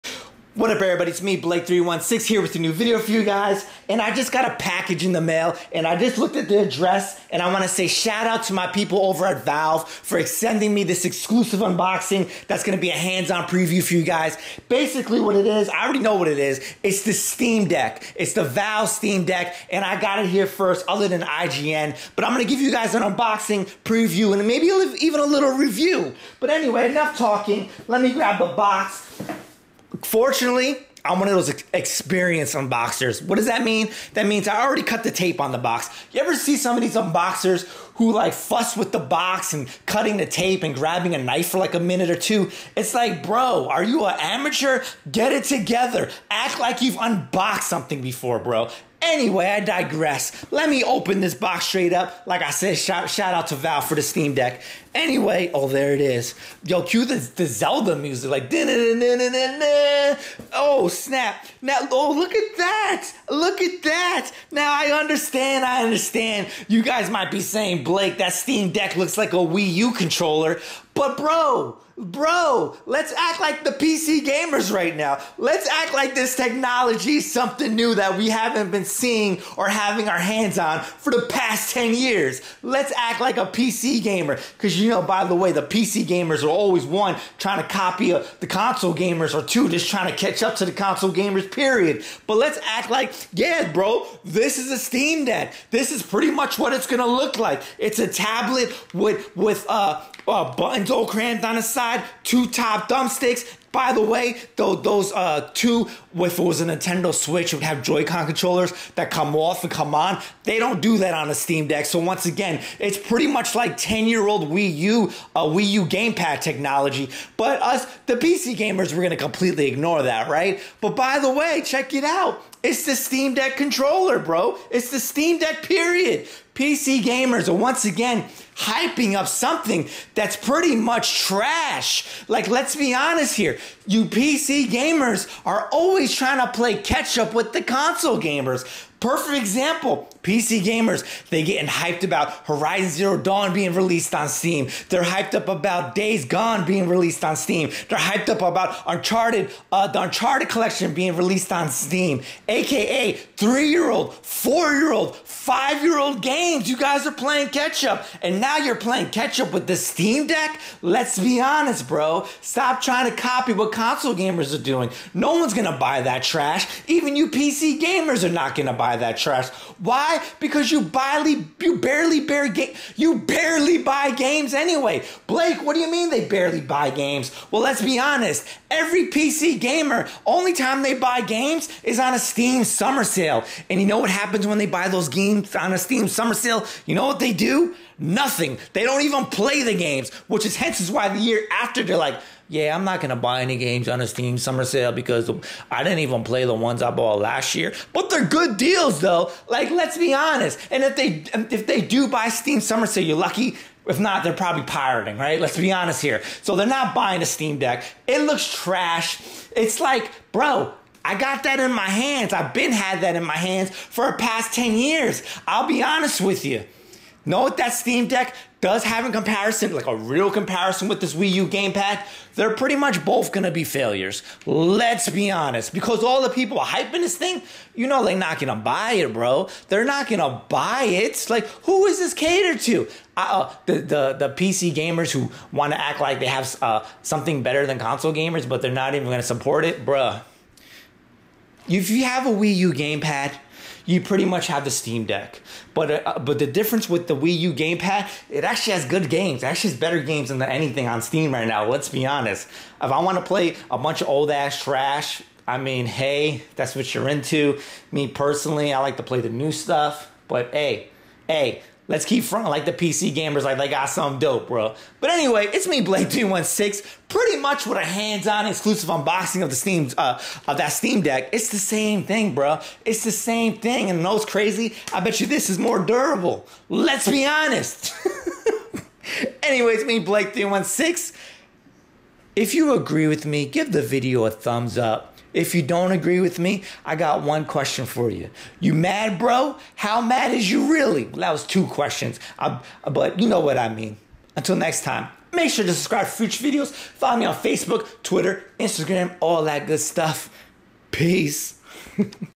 Exclusive Valve STEAM DECK UNBOXING!!!
Today we take an exclusive look with the newly announced Valve Steam Deck